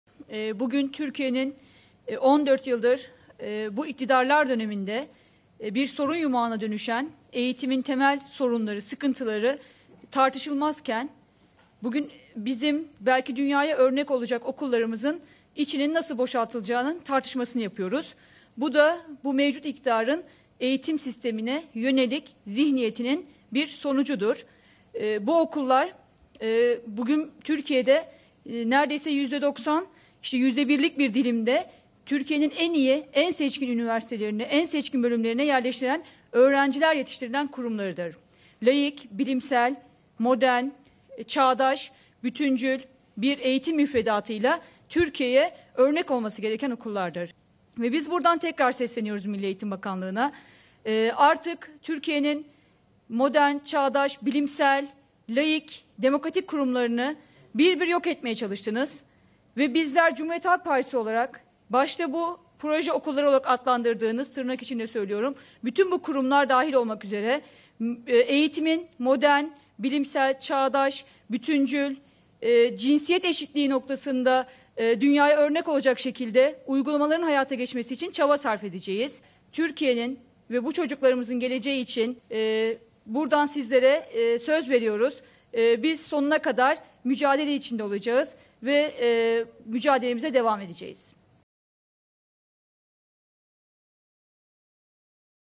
Eğitimde Mücadelemiz Devam Edecek!
İstanbul Milletvekilimiz Sibel Özdemir: Artık Türkiye'nin modern, çağdaş, bilimsel, laik, demokratik kurumlarını bir bir yok etmekten vazgeçin!